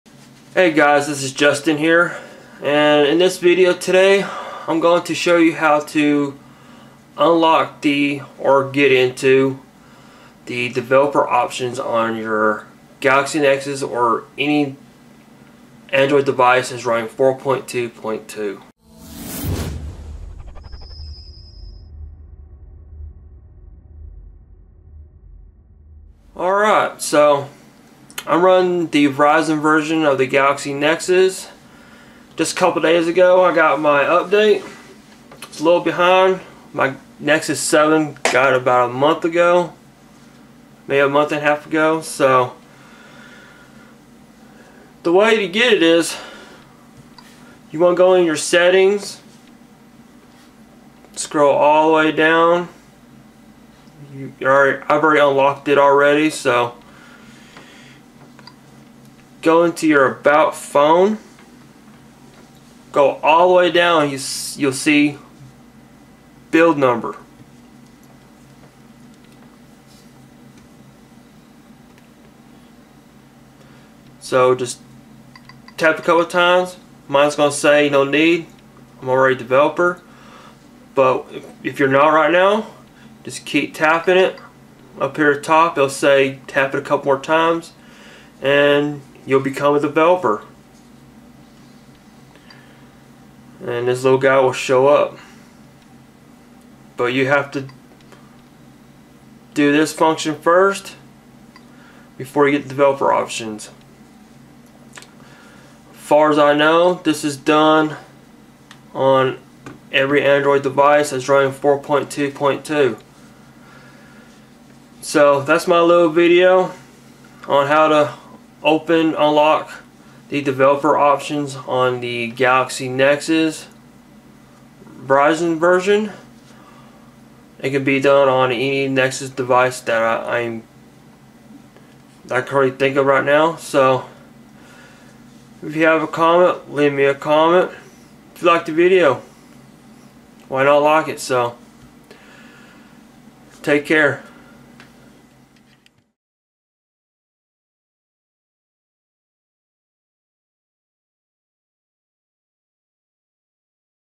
Verizon Galaxy Nexus 4.2.2 Developer Options
How to gain access to the Developer Options in Android 4.2.2 on the Verizon Wireless Galaxy Nexus.